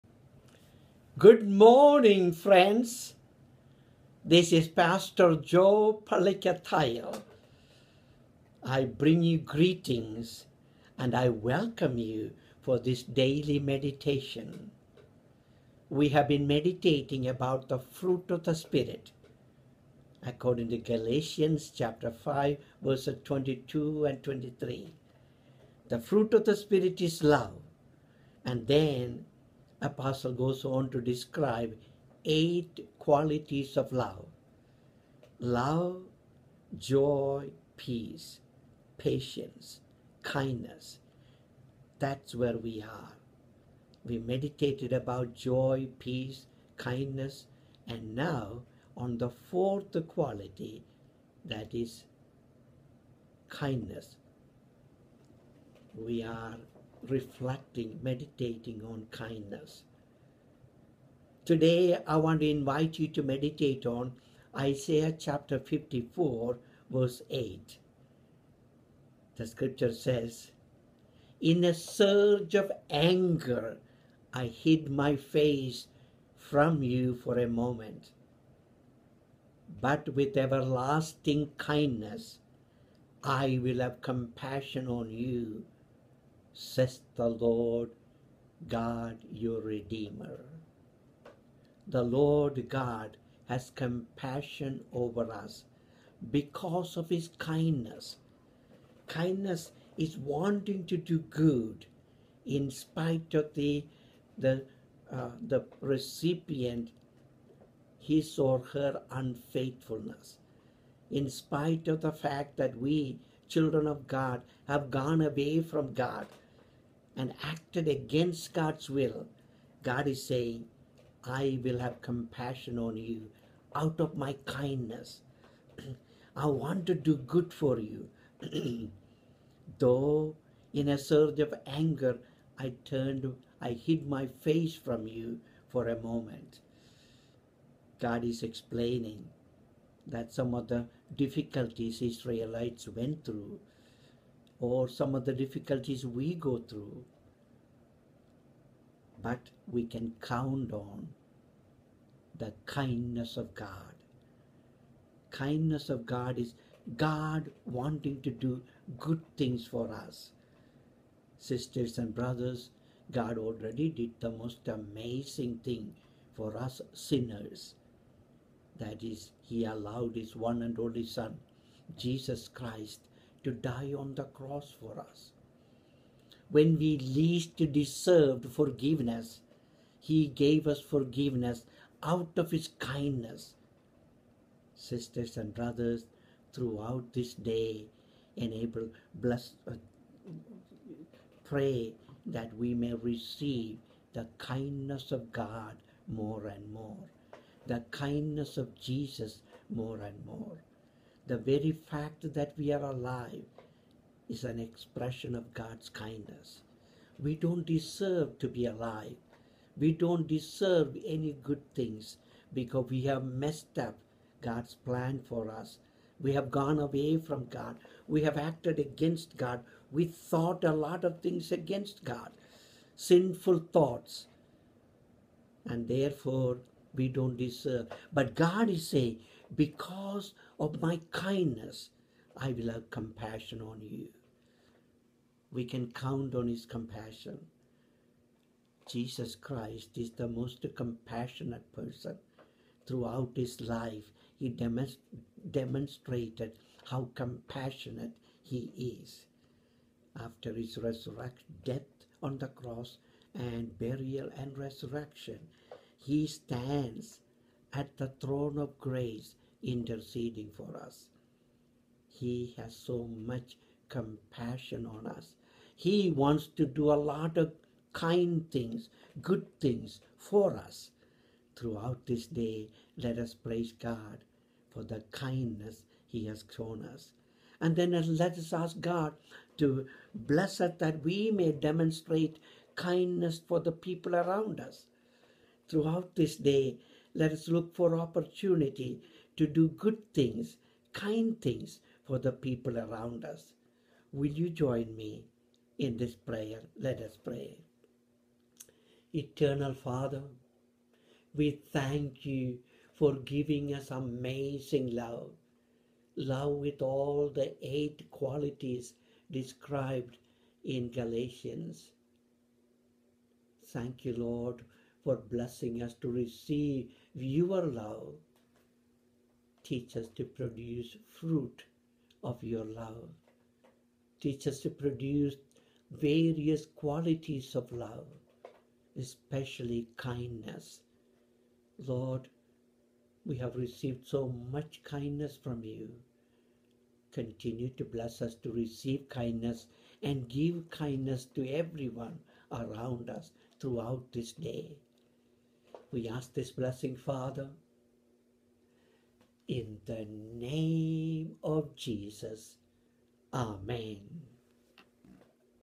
"In a surge of anger I hid my face from you for a moment, but with everlasting..."Isa.54:8.
via YouTube Capture:"In a surge of anger I hid my face from you for a moment, but with everlasting kindness I will have compassion on you, says the Lord your Redeemer." Isa. 54:8. The greatest kindness God expressed to humanity is the sacrifice of Jesus on the cross for our salvation. Let us continue to receive the kindness of God and give it to others.